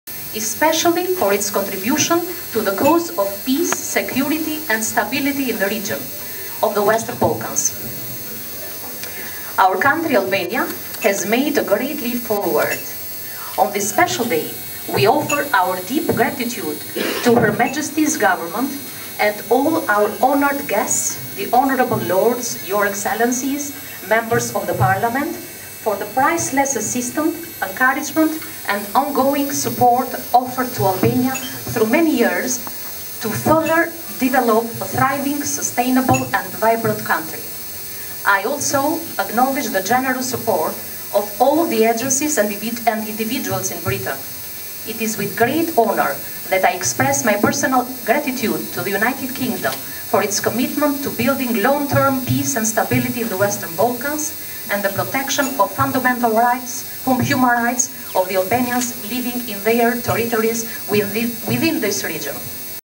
Teuta Starova of London Albania Embassy addressing guests, 28.11.2011
Teuta Starova of London Albania Embassy addressing guests during 28th November 2011, Albanian National Flag day